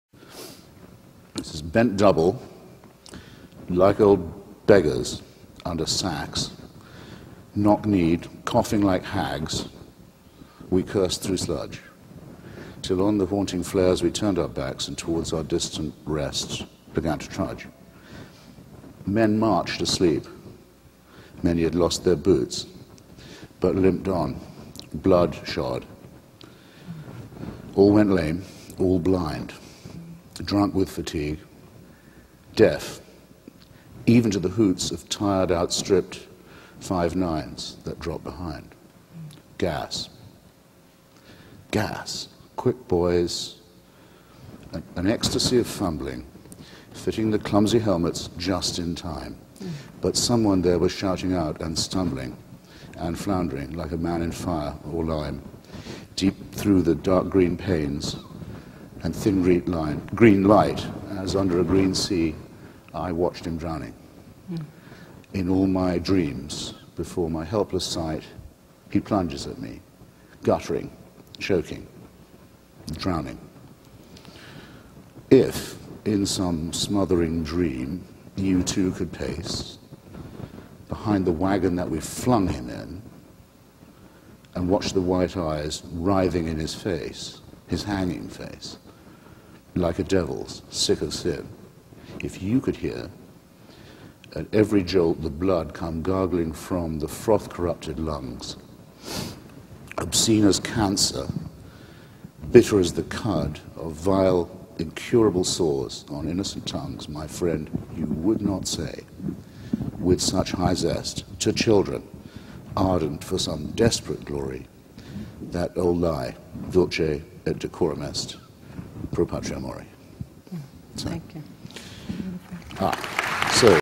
Wilfred Owen - Dulce et Decorum est - Recited by Christopher Hitchens
Wilfred Owen's poem, Dulce et decorum est.
Recited by Christopher Hitchens